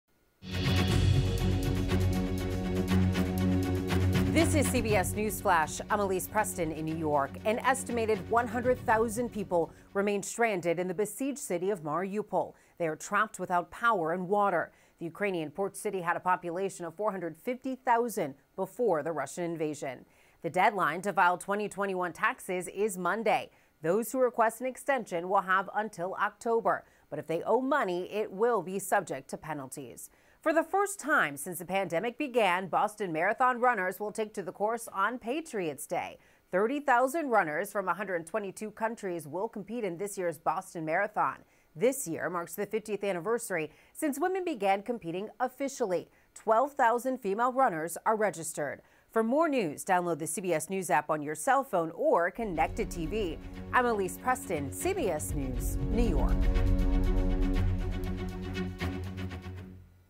Some 100,000 still stranded in besieged Mariupol
An estimated 100,000 people remain stranded without power and water in the besieged Ukrainian port city of Mariupol. Today is tax filing deadline day. And for the first time since the pandemic began, the Boston Marathon will be held on Patriot’s Day today.